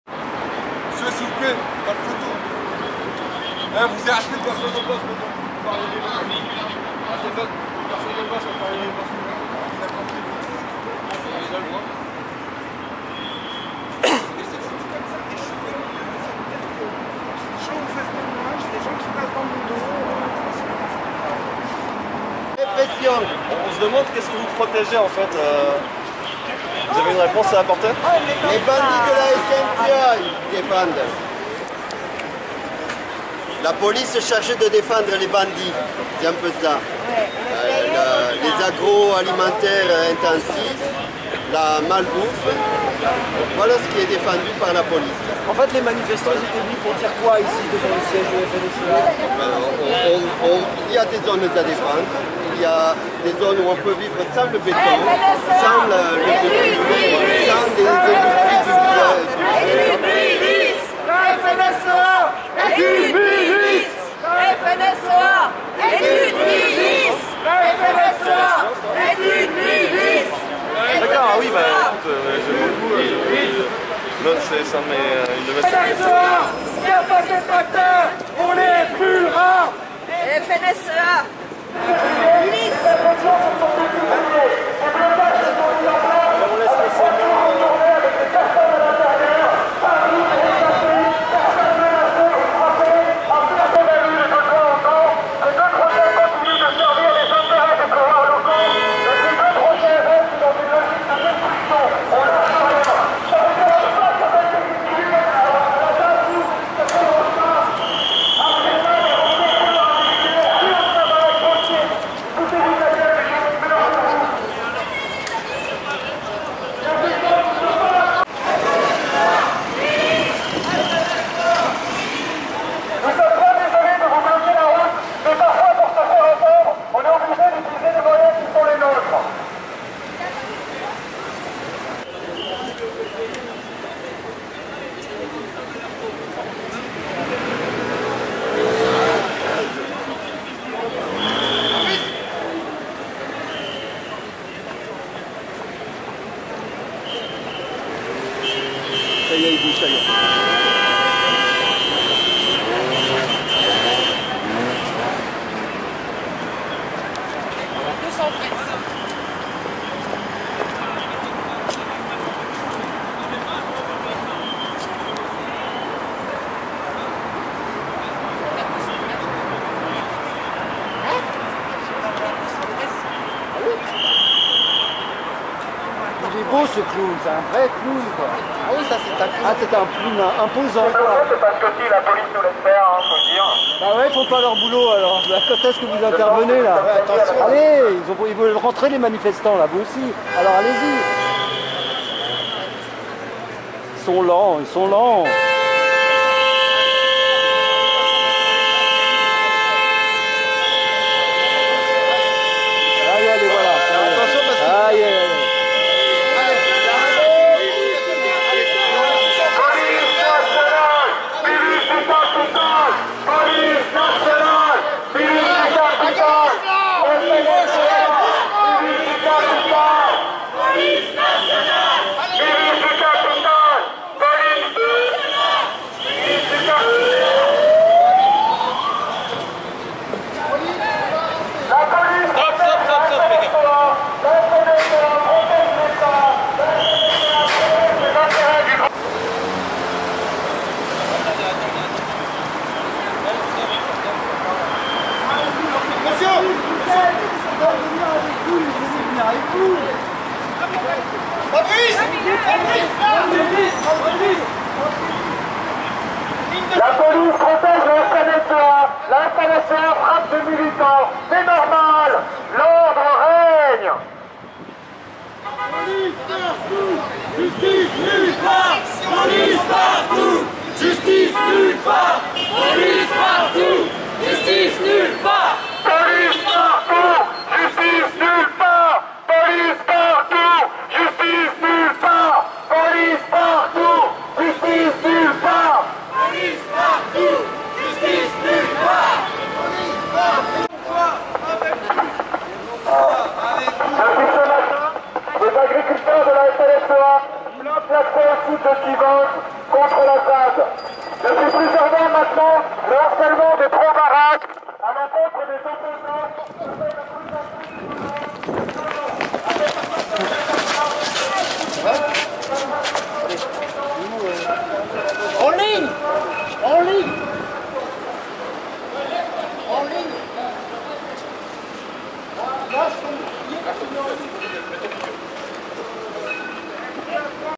Le siège de la FNSEA
Lundi 2 mars, quelques dizaines de militants se sont retrouvés au coin de la rue de la Baume, bloquée par la police, à quelques dizaines de mètres du siège de la FNSEA.

Les manifestants ont bloqué la circulation avant l'intervention des policiers. Après quelques minutes de marche, suivis par quelques gardes du corps, les manifestants ont gagné l'autre entrée de la rue de la Baume, elle aussi bloquée.

Les policiers ont tenté d'encercler le groupe de soutiens aux zadistes, mais sans succès. Pour éviter des interpellations, les manifestants ont formé un groupe compact pour rejoindre le métro le plus proche.